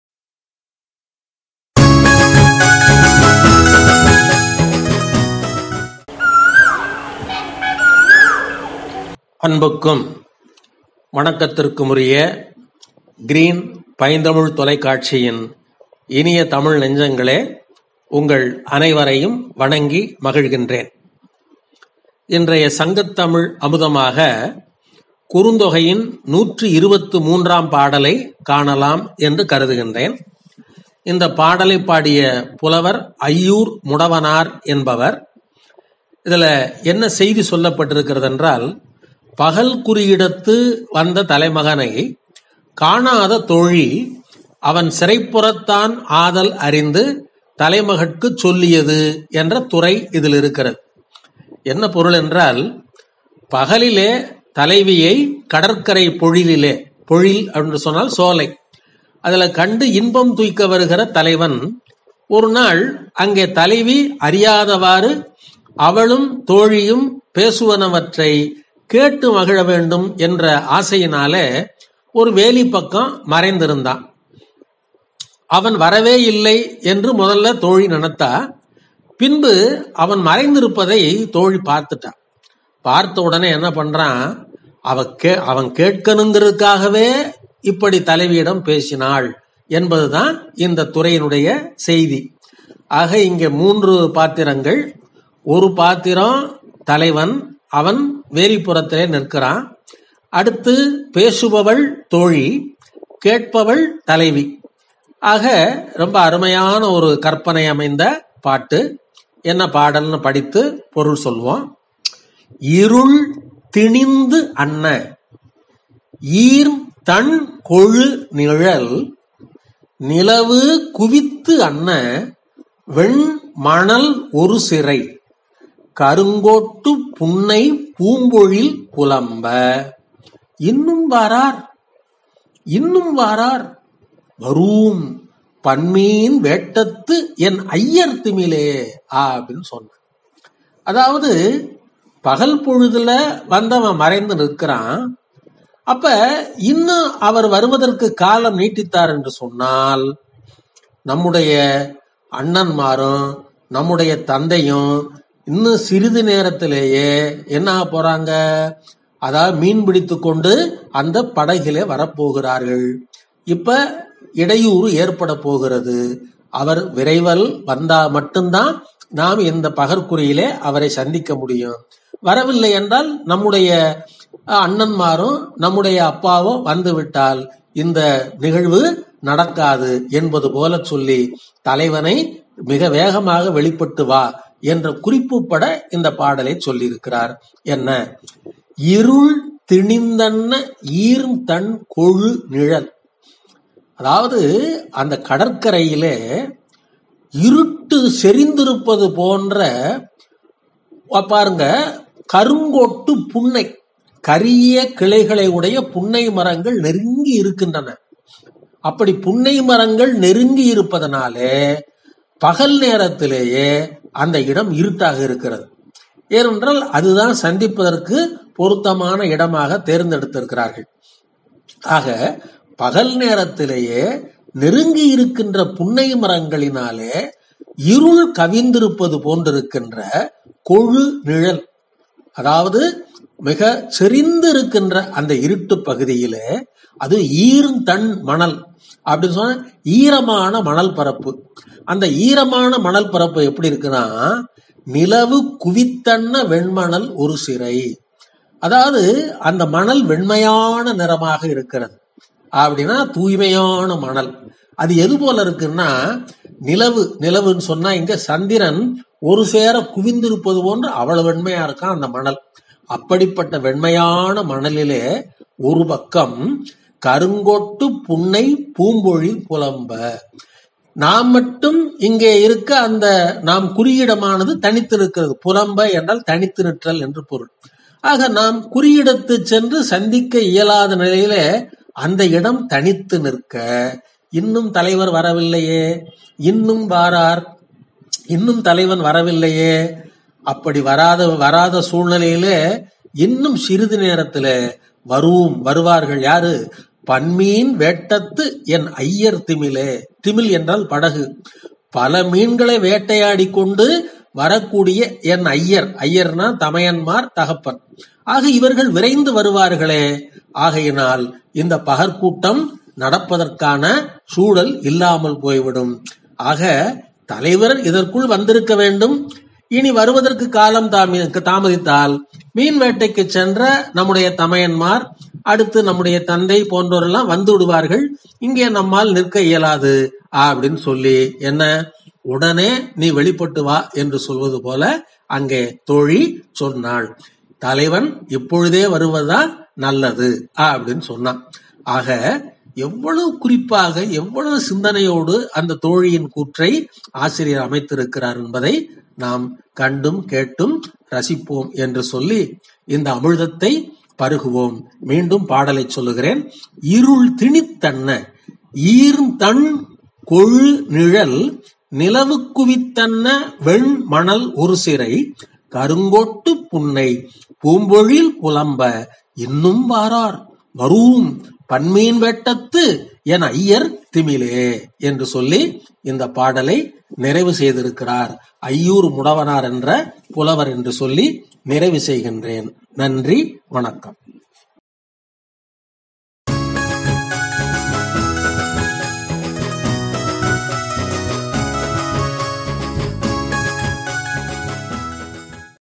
சங்கத்.தமிழ் அமுதம் - குறுந்தொகை - பாடல் 123 -, ஐயூர் முடவனார் எழுதிய பாடல் - Green Tamil TV.
சங்க இலக்கியம் - எட்டுத்தொகை - குறுந்தொகை - பாடல் 123 - ஐயூர் முடவனார் - பாடல் விளக்கம் - பெரும்புலவர்.திரு.மு.சன்னாசி அவர்கள்greentamiltv குறுந்தொகைபாடல்123#